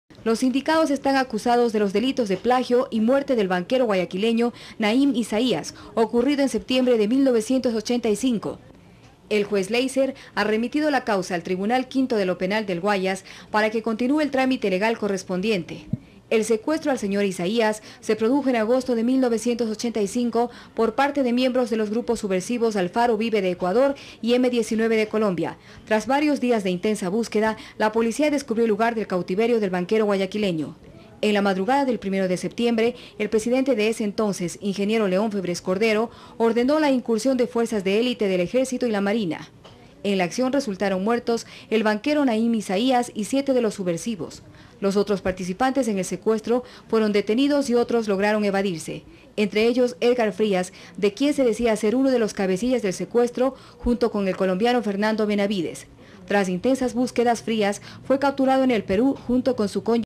Secuestro del Banquero Nahím Isaías a manos de Alfaro Vive Carajo- 1985
🟡 Archivo Audiovisual del Ecuador 🟡

Descripción:

✨ El banquero Nahim Isaías, gerente general de Filanbanco,  estuvo cautivo por 26 días en una casa de La Chala en Guayaquil.

🟡 El 2 de septiembre, en el operativo de rescate murieron el banquero y todos los secuestradores del grupo subversivo AVC.